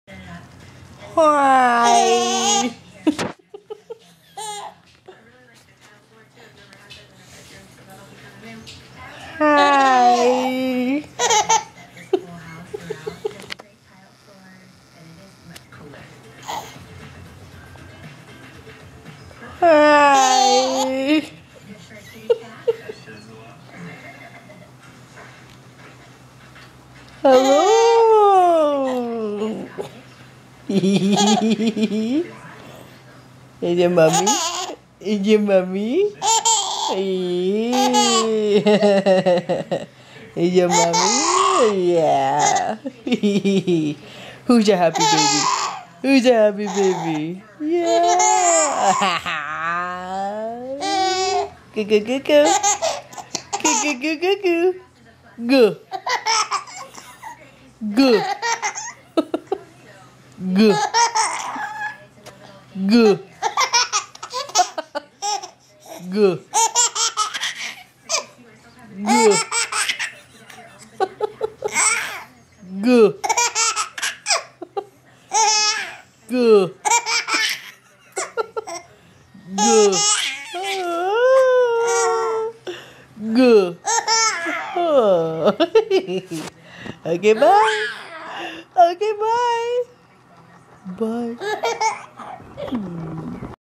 Laughy Teddy
Mommy making Teddy laugh